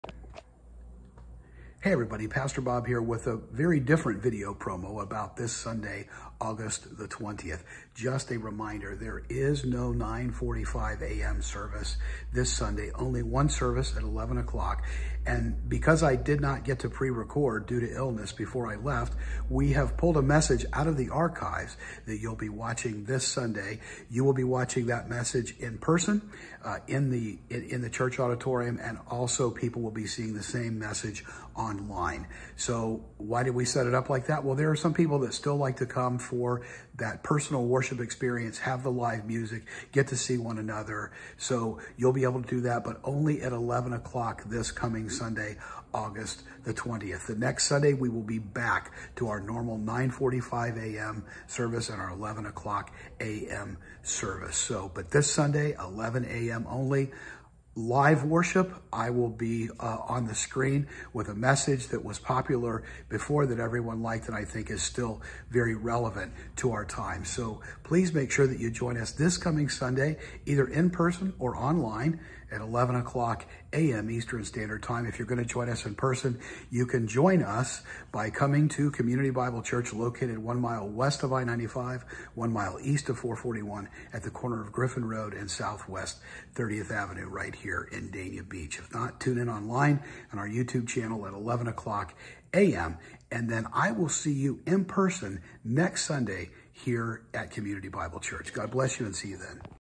All About Sunday August 20, 2023
And this brief video Pastor Bob talks about what’s upcoming on Sunday, August 20, 2023. ￼